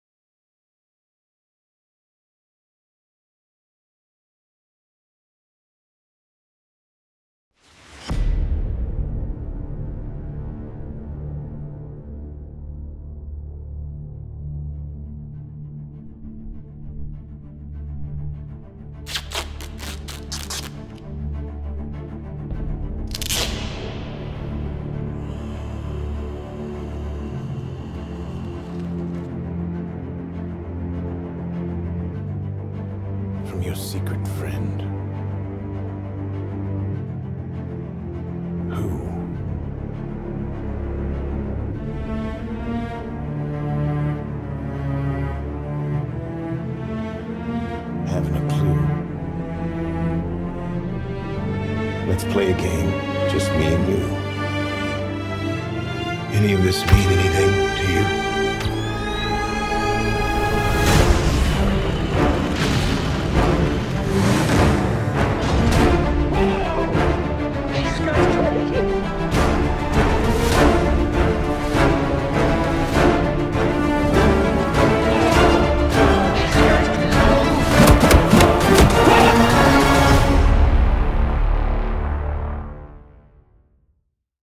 The Batman 2021 (Fan Made Trailer)
What surprises will this film have in store for us?

I made this little trailer with original music composed by me and published on AudioJungle.

Simone Liotta - SIAE, CAE/IPI number : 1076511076